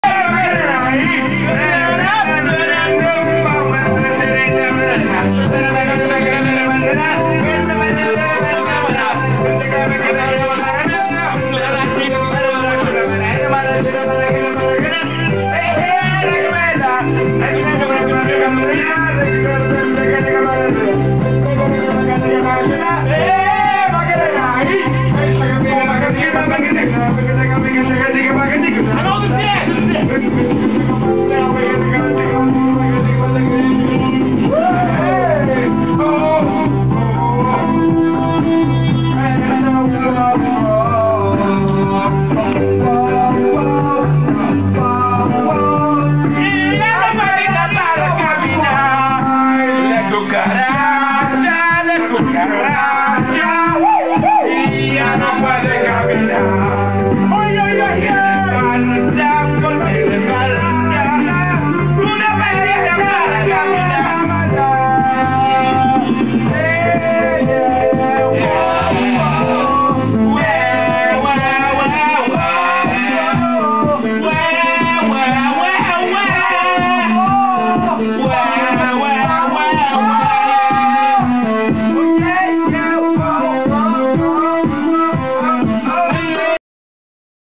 jews trying to dance
a jewish dance drunk kids in school dancing